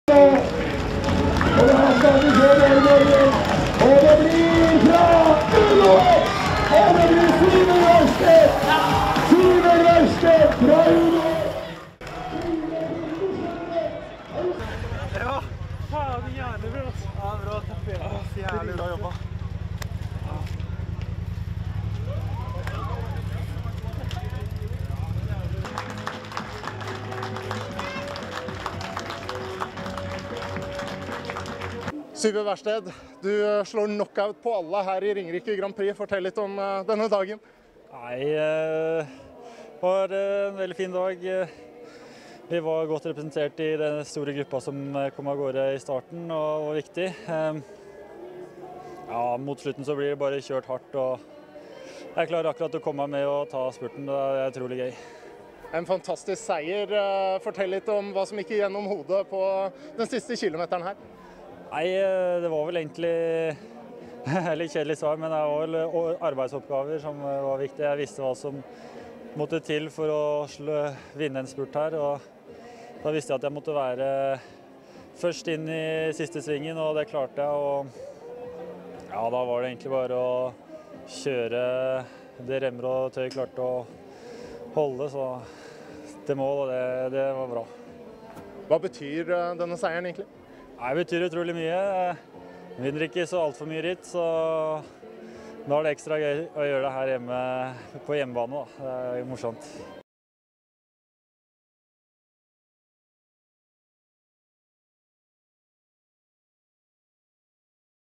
Syver Westgaard Wærsted - Ringerik Grand Prix
Syver Westgaard Wærsted knuste all motstand og tok en etterlengtet seier for Uno-X Norwegian Development Team under Ringerike Grand Prix.
Dette rittet er selveste oppladningen for de norske lagene som skal delta under Tour of Norway.